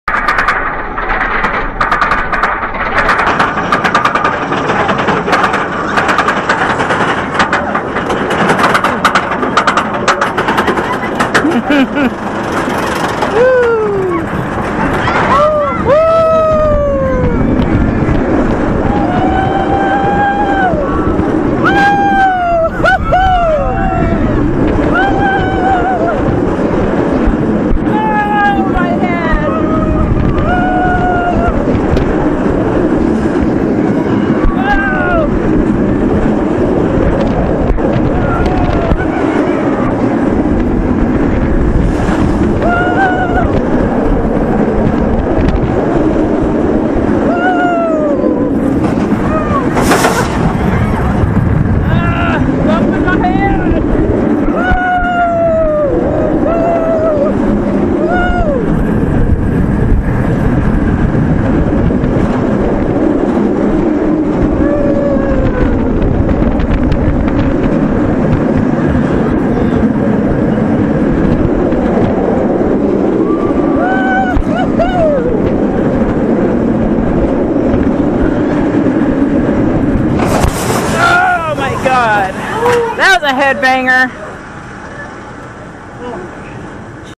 POV | The Riddler's Revenge | Six Flags Magic Mountain!!!
A little shaky & quite the head banger but still a fun ride 👍